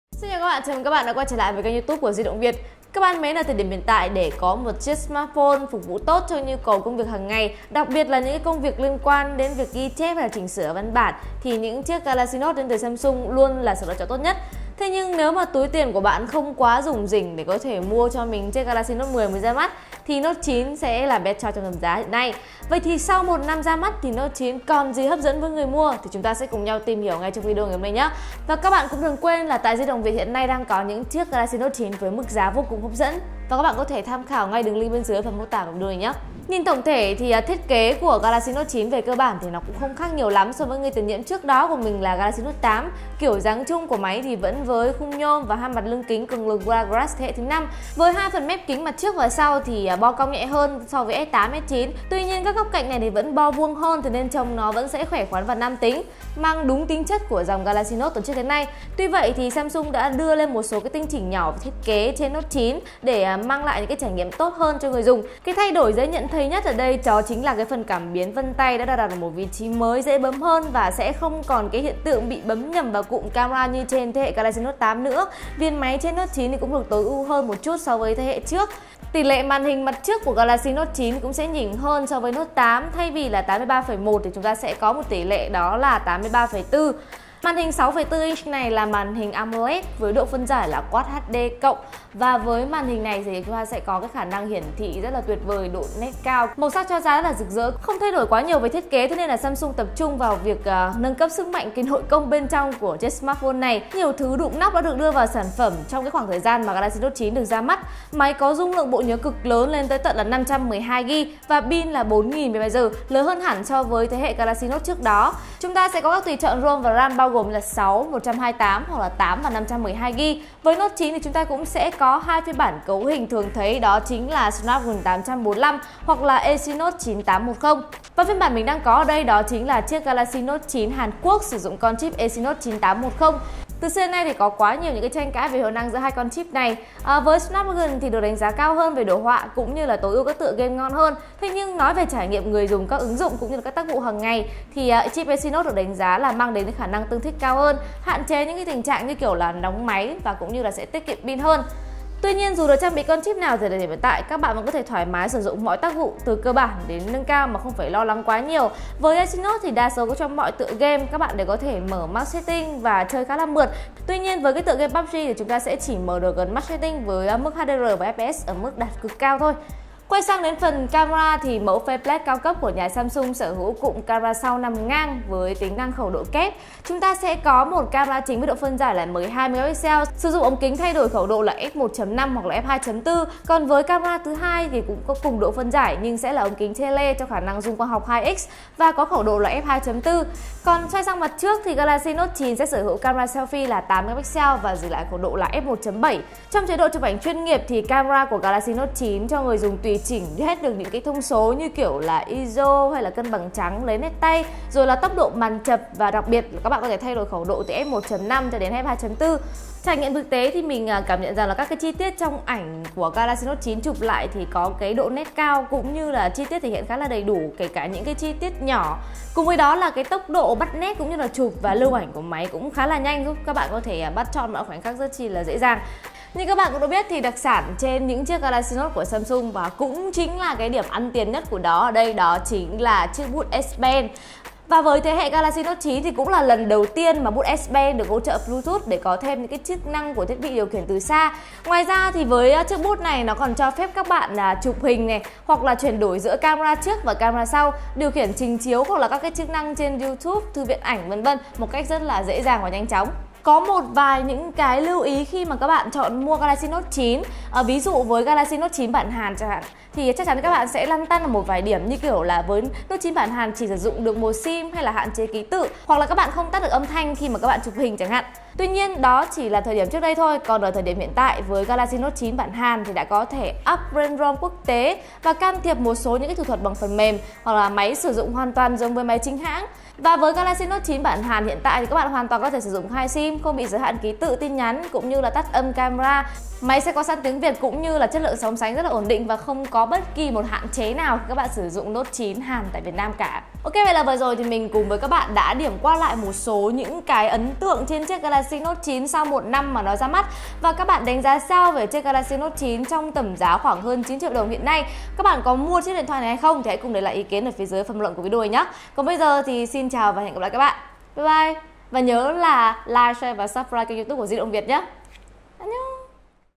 Sau 1 năm Note 9 còn đáng mua không?
Chi tiết về sản phẩm trong video các bạn có thể tham khảo tại:

Galaxy Note 9 đã có mặt 1 năm trên thị trường, thông thường những chiếc điện thoại Android sau 1-2 năm thường được cho là sẽ bị lag và xuống cấp nhanh hơn so với iPhone. Đặc biệt là với Galaxy Note 9 bản Hàn chạy con chip Exynos thì sự hoài nghi lại càng lớn. Vậy thực tế Galaxy Note 9 Hàn có bị lag sau 1 năm?